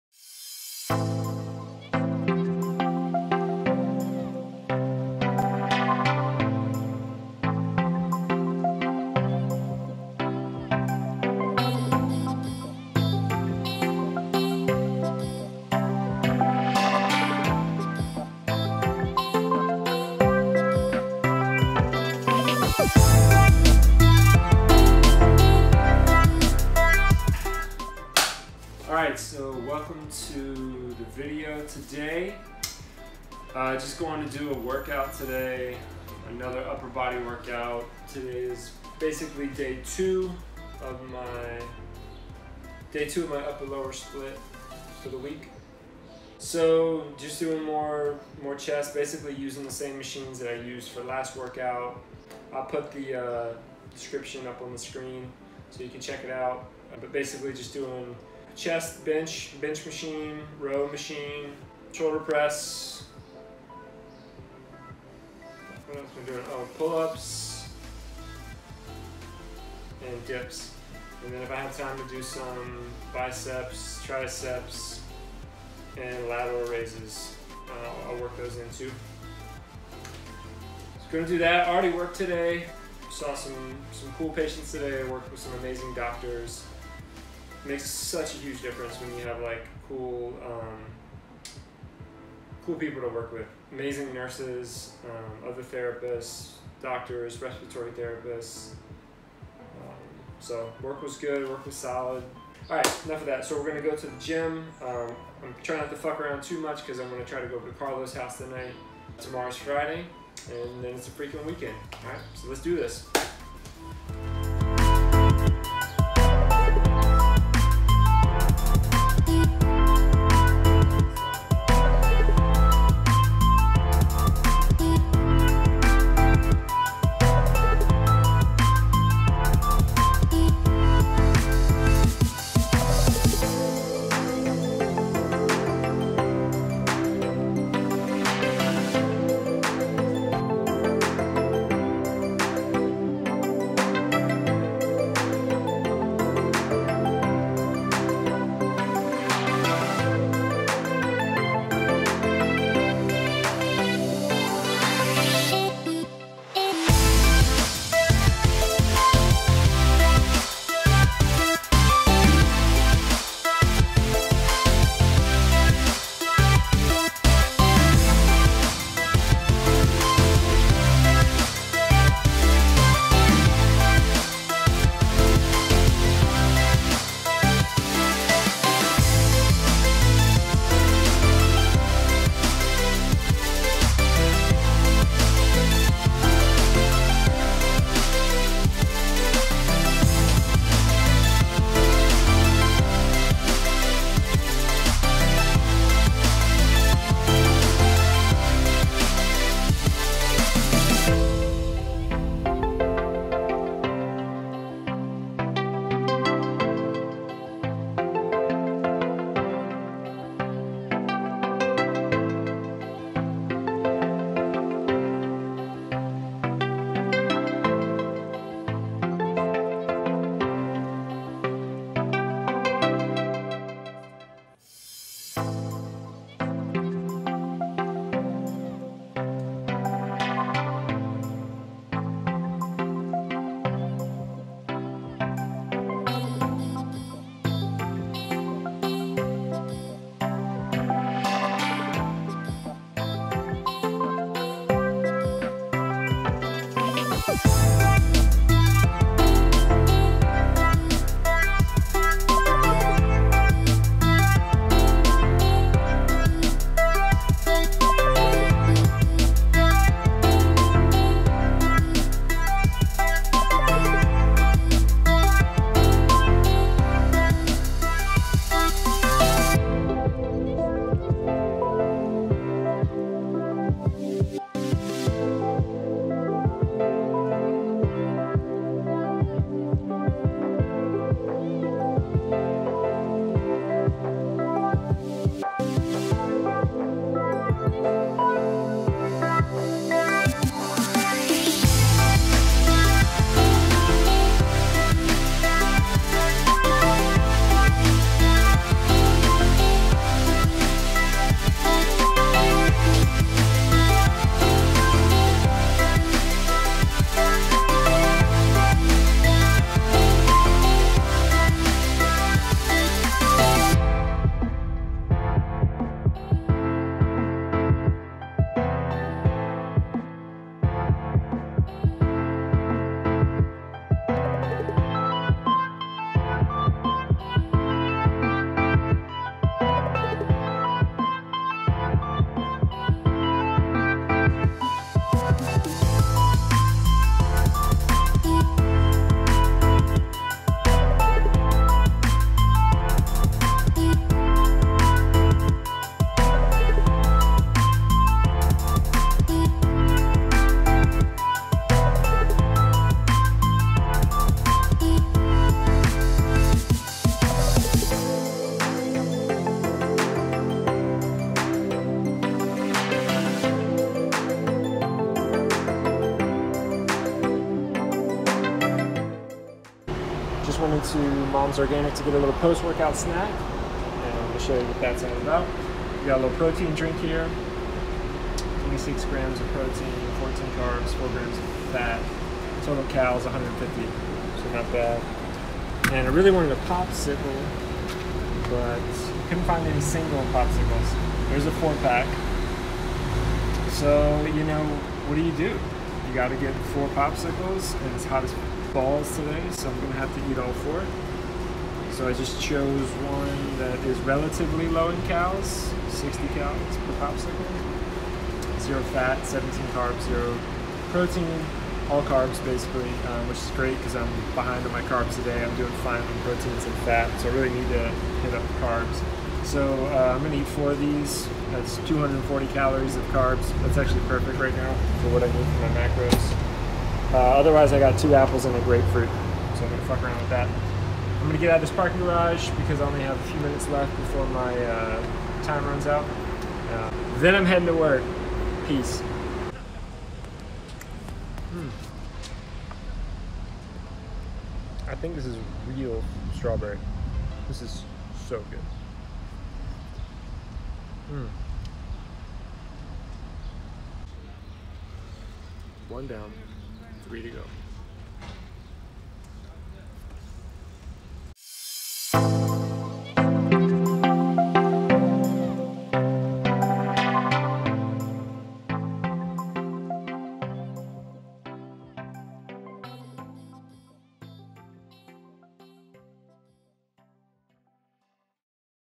Strawberry Feels--Summer Vlog #2
Hitting the gym for an upper body session, pigging out on some delicious frozen strawberry pops, and a clue about Panchito's past life (but still no excuse for his shoddy camera work).

My go to whey protein: Ascent Native Whey - Chocolate

(shipping is always free)